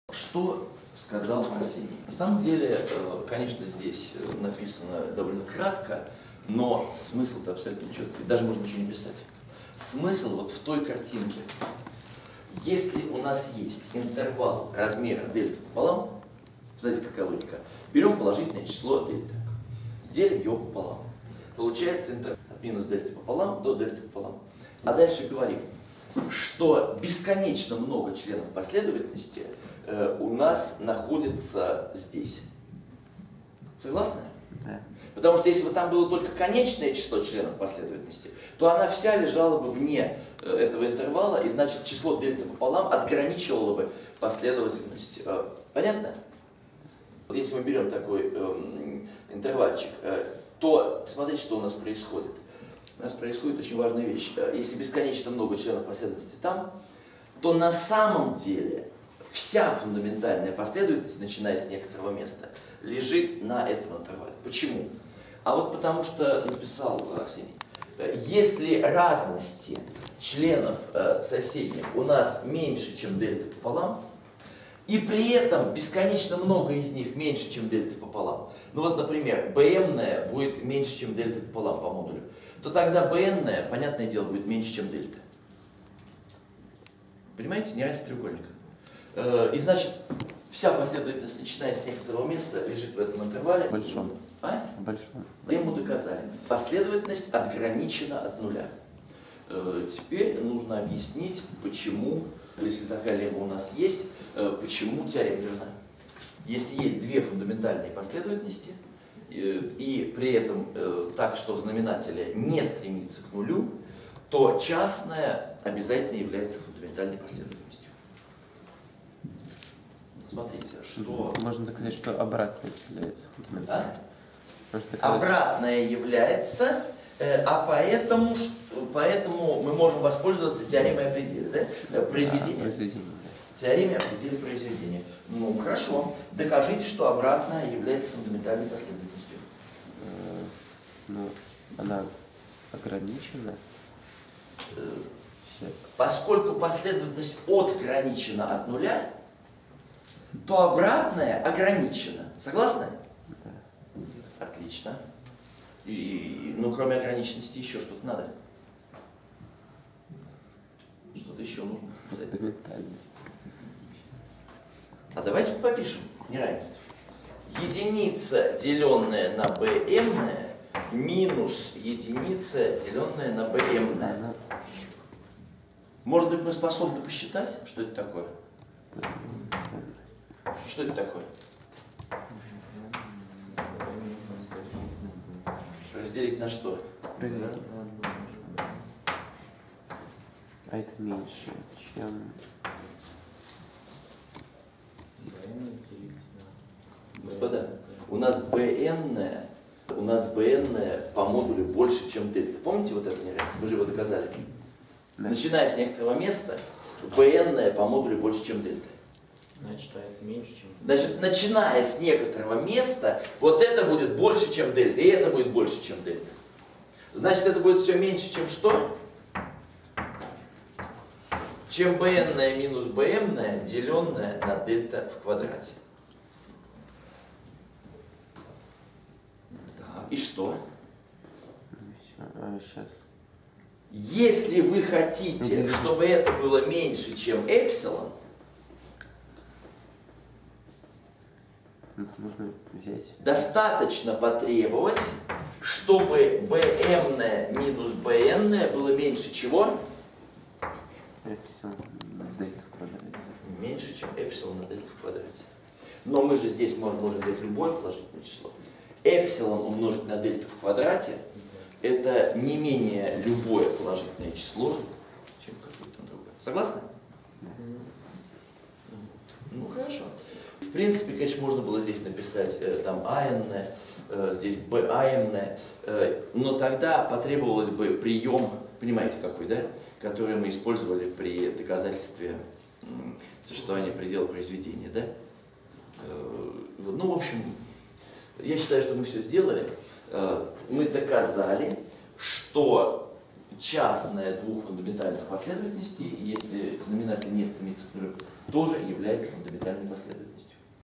Частное двух сходящихся в себе последовательностей
Частное двух сходящихся в себе последовательностей сходится в себе, если знаменатель не стремится к нулю.